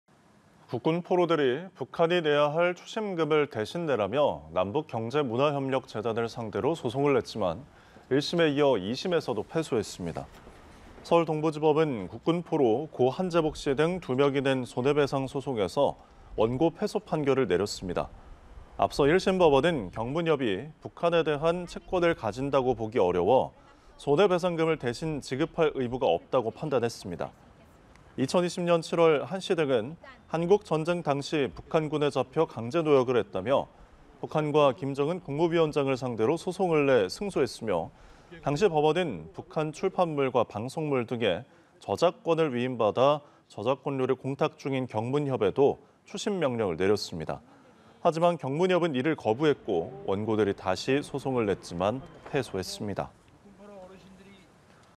"북한 저작권료로 배상금 달라"…국군포로 2심도 패소 / 연합뉴스TV (YonhapnewsTV)
"북한 저작권료로 배상금 달라"…국군포로 2심도 패소

국군포로들이 북한이 내야 할 추심금을 대신 내라며 남북경제문화협력재단을 상대로 소송을 냈지만 1심에 이어 2심에서도 패소했습니다.

서울동부지법은 국군포로 고 한재복 씨 등 2명이 낸 손해배상 소송에서 원고 패소 판결을 내렸습니다.

앞서 1심 법원은 경문협이 북한에 대한 채권을 가진다고 보기 어려워 손해배상금을 대신 지급할 의무가 없다고 판단했습니다.

2020년 7월 한 씨 등은 한국전쟁 당시 북한군에 잡혀 강제노역을 했다며 북한과 김정은 국무위원장을 상대로 소송을 내 승소했으며 당시 법원은 북한 출판물과 방송물 등의 저작권을 위임받아 저작권료를 공탁 중인 경문협에도 추심 명령을 내렸습니다.

하지만 경문협은 이를 거부했고, 원고들이 다시 소송을 냈지만 패소했습니다.